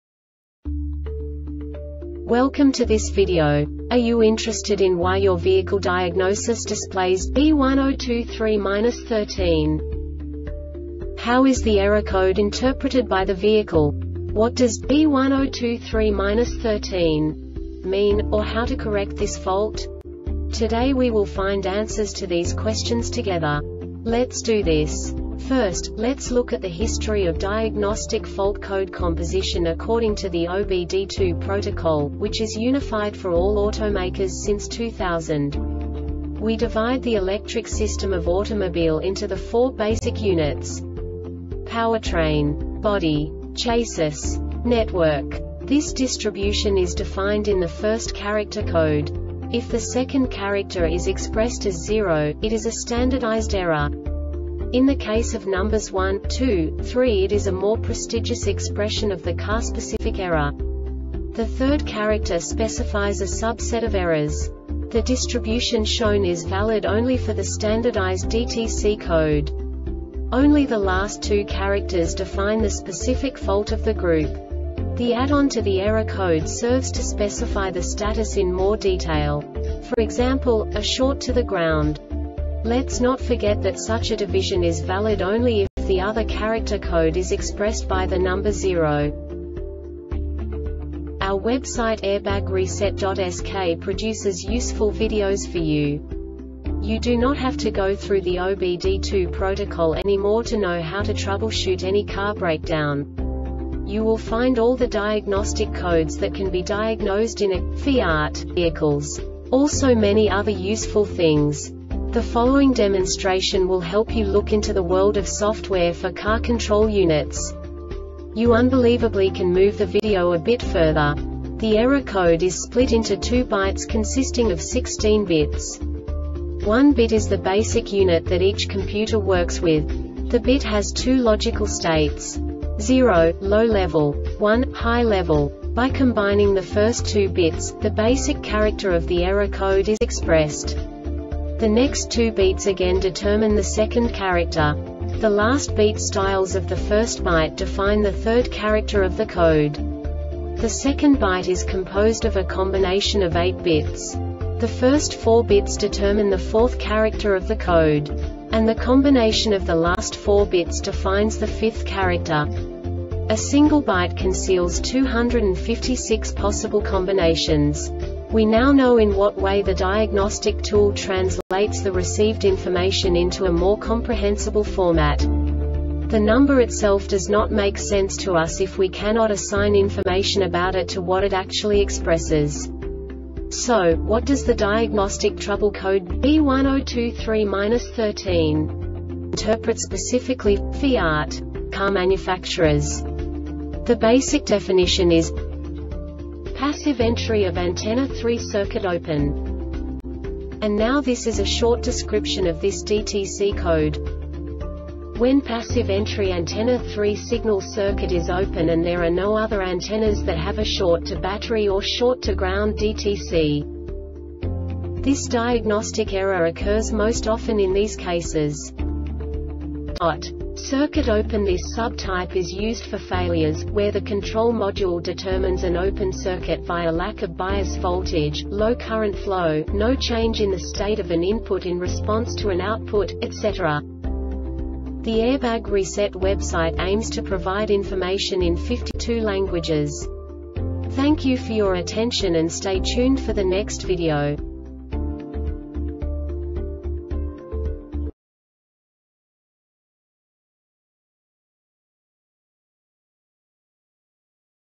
DTC Fiat B1023-13 Short Explanation
Contents:
0:21 Basic DTC analysis according to OBD2 protocol standard.
1:48 Insight into programming
2:58 A diagnostically understandable description of the Diagnostic Trouble Code
3:05 Basic error definition

Make:
FIAT
Code:
B1023-13
Definition:
Passive Entry Of Antenna 3 - Circuit Open
Description:
When Passive Entry Antenna 3 Signal circuit is Open and there are no other Antennas that have a Short to Battery or Short to Ground DTC.
Failure Type:
Circuit Open